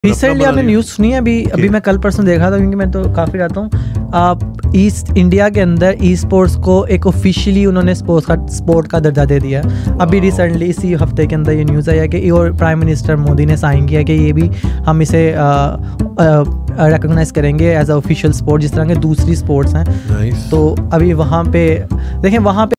Arslan Ash Champion Talking About Indian Gov On Gaming | Junaid Akram | Podcast
In this video arslan ash the recently world champion of tekken 7 is talking about Indian government about the gaming in Junaid akram podcast